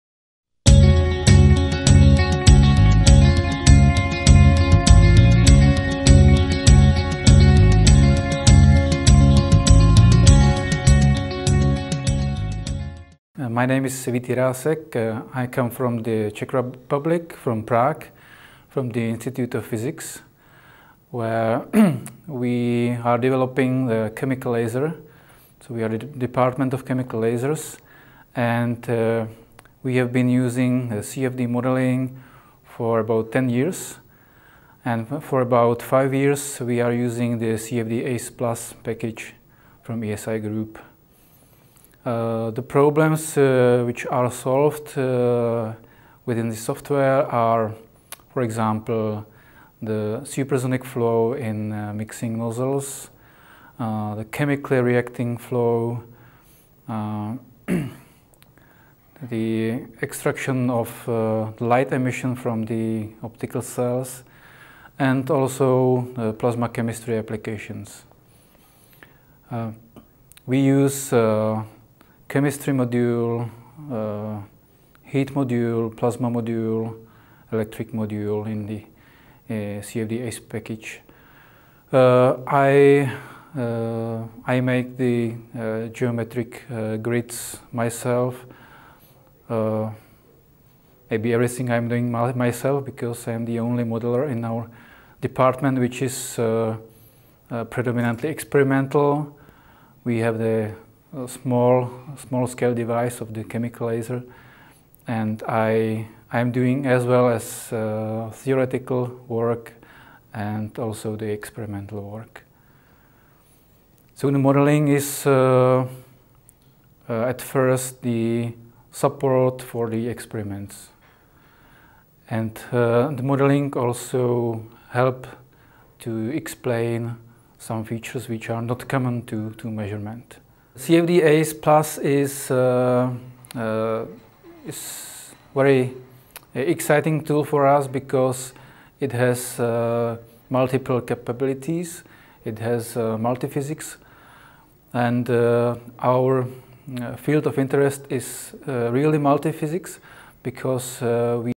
CFD-ACE+ customer testimonial: Vit Jirasek, Institute of Physics
Vit Jirasek, Project Engineer in the Department of Gas Lasers at the Institute of Physics in Czech Republic, explains which problems thye can solve using CFD modelling with CFD-ACE+.